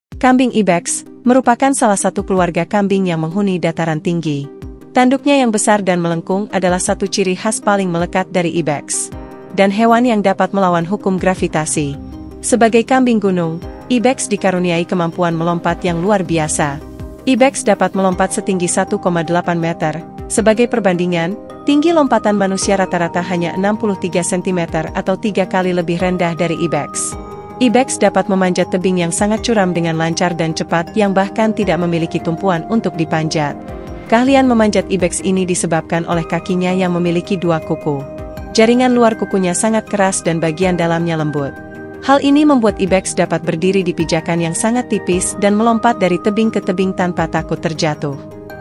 Melawan Hukum Gravitasi
Kambing ibex, merupakan salah satu keluarga kambing yang menghuni dataran tinggi. Tanduknya yang besar dan melengkung adalah satu ciri khas paling melekat dari ibex.